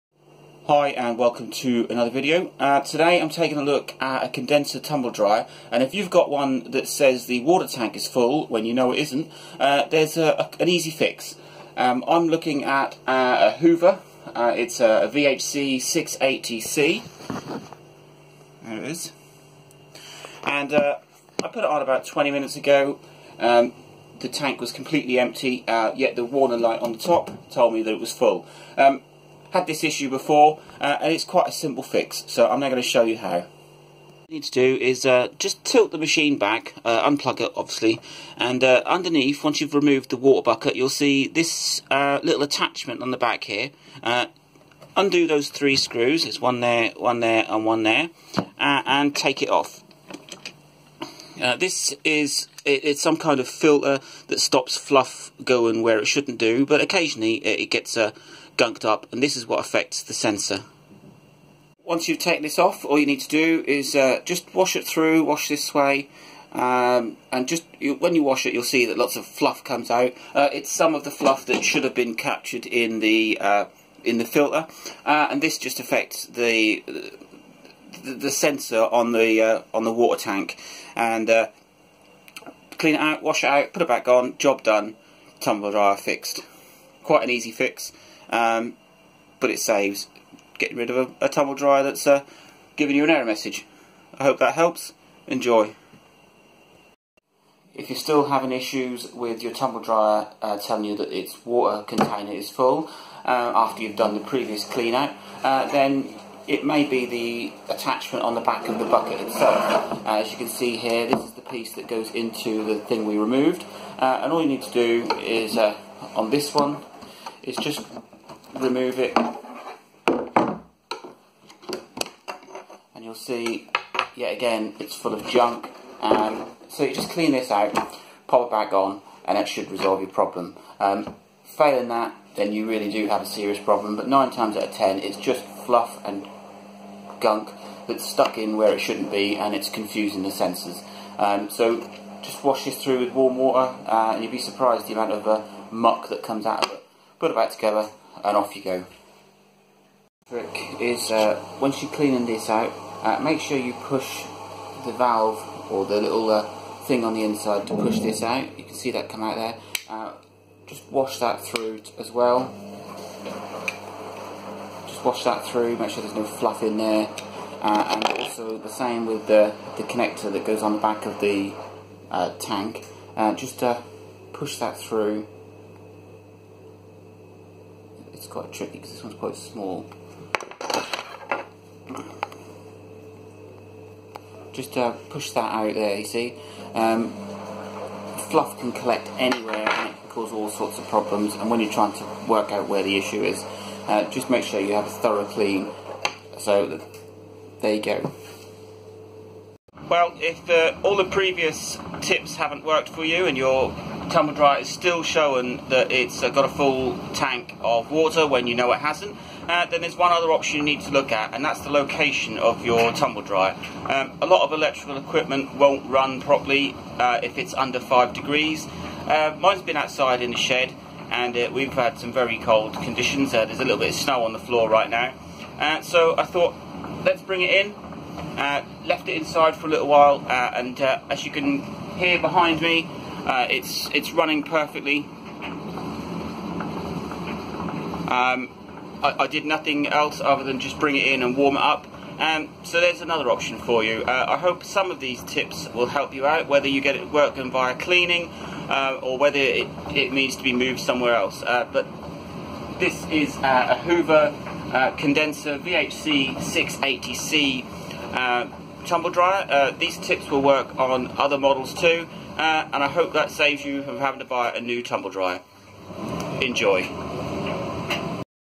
Hoover VHC 680C Tumble Dryer. Water Tank Full Error. Fixes. Tumble Dryer Fix.
If you have a Hoover VHC 680C condenser tumble dryer which refuses to work, claiming the water tank is full then these fixes may help you. It can also help with other tumble dryers too.